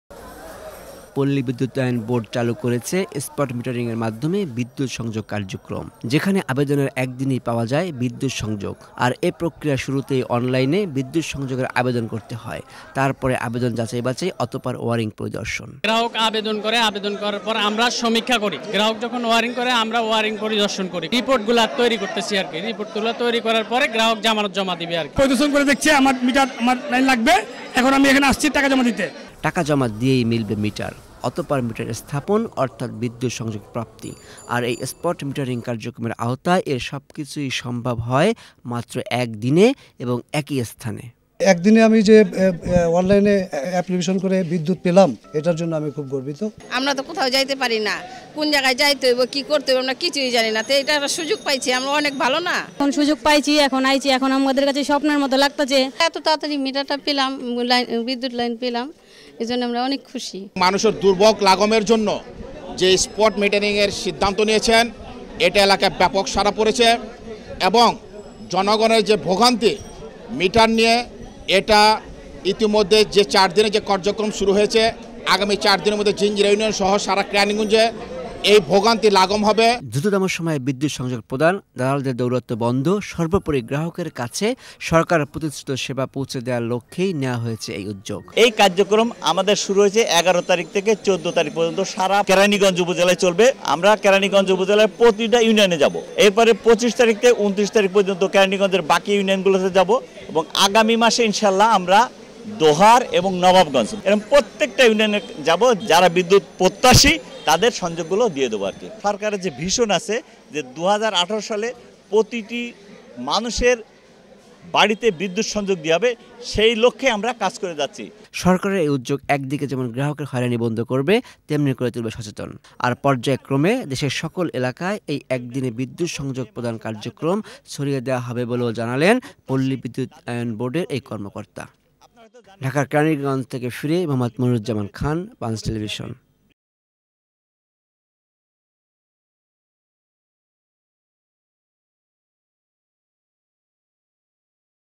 Electricity in Bangladesh (REB Spot Metering Program)
Production of Electricity has been increased tremendously in Bangladesh as well as new connection. Rural Electrification Board REB has initiated new program which called Spot Metering Program under this program people will get new connection within one day.